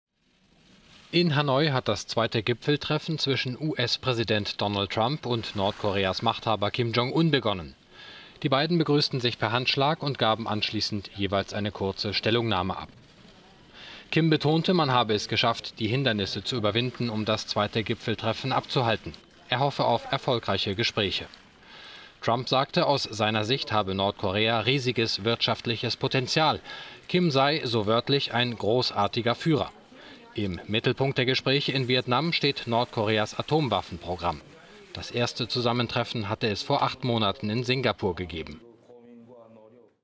Kim und Trump: Handschlag und warme Worte
In Hanoi hat das zweite Gipfeltreffen zwischen US-Präsident Donald und Nordkoreas Machthaber Kim Jong Un begonnen.…